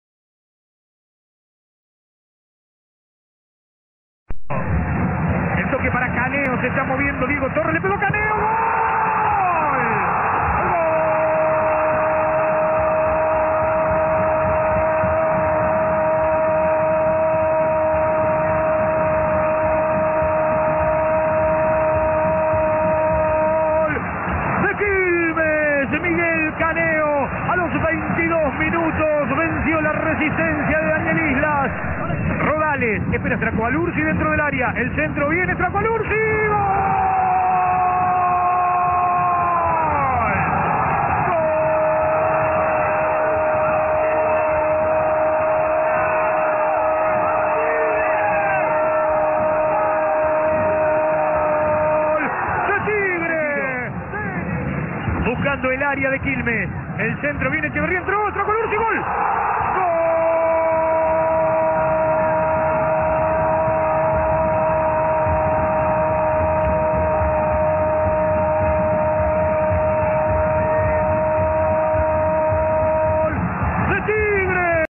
Quilmes 1-2 Tigre 6/03/2011
Goals & Highlights
Argentina Primera Division
7/03/2011
Quilmes 1-2 Tigre